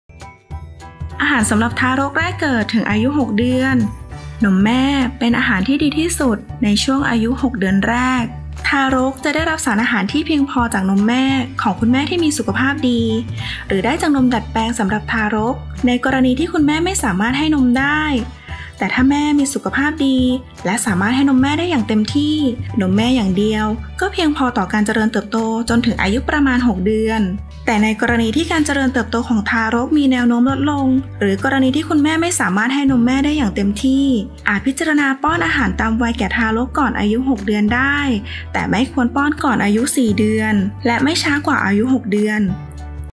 อาหารตามวัยทารก สำหรับทารกแรกเกิด ถึงอายุ 6 เดือน | โรงพยาบาลนครธน
🔸 อาหารสำหรับทารกแรกเกิด ถึงอายุ 6 เดือน 🔸

นมแม่ เป็นอาหารที่ดีที่สุดในช่วงอายุ 6 เดือนแรก ทารกจะได้รับสารอาหารที่เพียงพอจากนมแม่ของคุณแม่ที่มีสุขภาพดี หรือได้จากนมดัดแปลงสำหรับทารก ในกรณีที่คุณแม่ไม่สามารถให้นมได้ แต่ถ้าแม่มีสุขภาพดี และสามารถให้นมแม่ได้อย่างเต็มที่ นมแม่อย่างเดียวก็พอเพียงต่อการเติบโตของลูกจนถึงอายุประมาณ 6 เดือน แต่ในกรณีที่การเจริญเติบโตของทารกมีแนวโน้มลดลง หรือกรณีที่คุณแม่ไม่สามารถให้นมแม่ได้อย่างเต็มที่ อาจพิจารณาป้อนอาหารตามวัยแก่ทารกก่อนอายุ 6 เดือนได้ แต่ไม่ควรป้อนก่อนอายุ 4 เดือน และไม่ช้ากว่าอายุ 6 เดือน
หากคุณพ่อคุณแม่มีคำถามเกี่ยวกับอาหารสำหรับทารกแรกเกิด สามารถปรึกษาพยาบาลวิชาชีพ แผนกเด็กแรกเกิด โรงพยาบาลนครธน
📞 สอบถามรายละเอียดเพิ่มเติมได้ที่ แผนกเด็กแรกเกิด ชั้น 5 โรงพยาบาลนครธน

📢 หากไม่อยากพลาดข่าวสารดี ๆ เกี่ยวกับสุขภาพ อย่าลืมกด ติดตาม' และ 'กดกระดิ่ง' 🔔 ด้วย ^^

📢 หรือติดตามสาระความรู้เรื่องสุขภาพจากโรงพยาบาลนครธน ในช่องทางอื่น ๆ ได้ที่